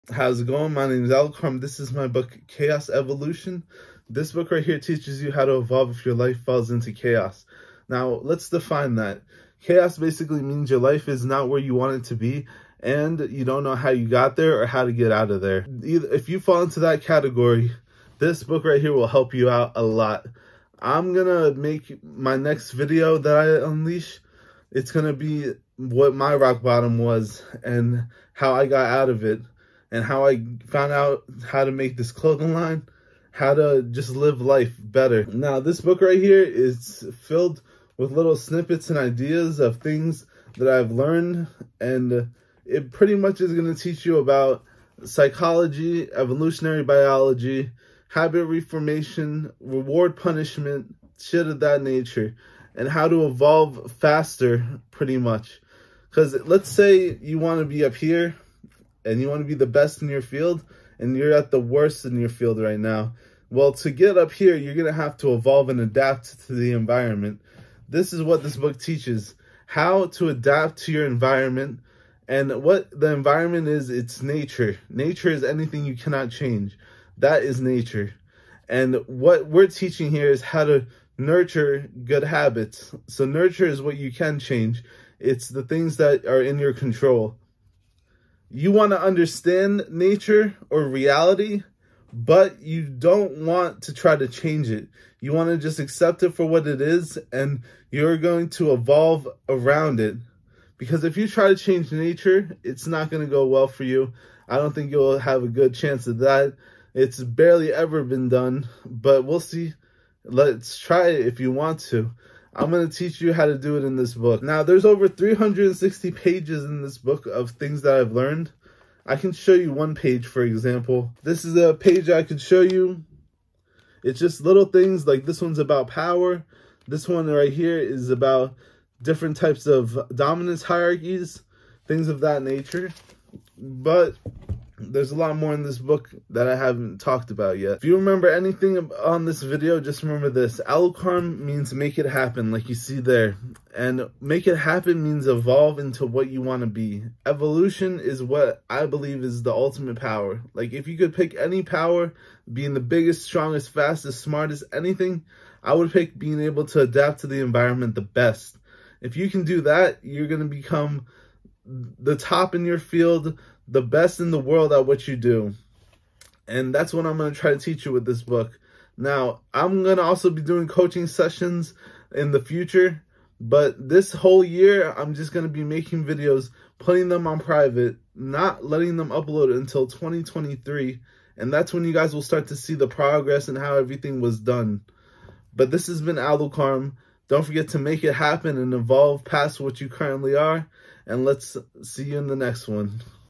Chaos EVOLUTION By Alucarm
This is the book I am writing chaos evolution how to evolve if your life is chaos right now. Learning nature/nurture, evolutionary biology, reward punishment, symbiotic evolution.